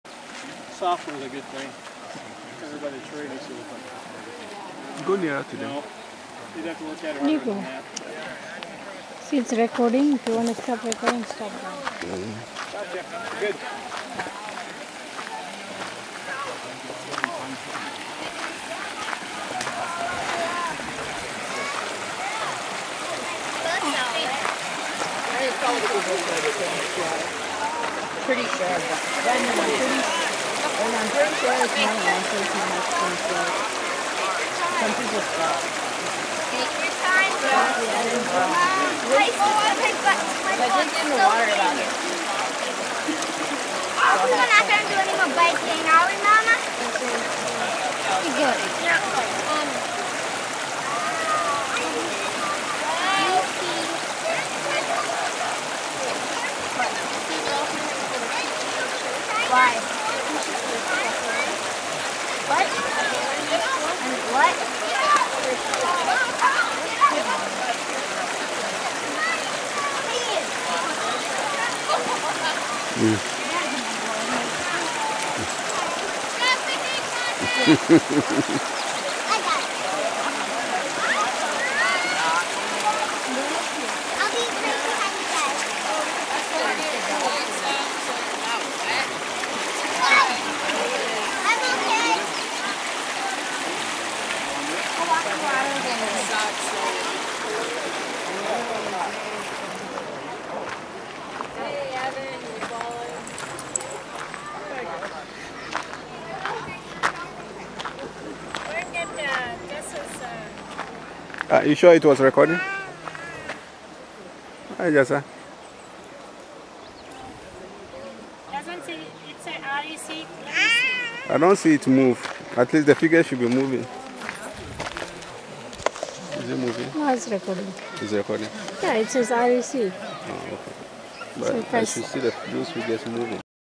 2012-07-08 Itaska Mississippi Headwaters
Ben and Rachael Leadholm walk across the headwaters of the Mississippi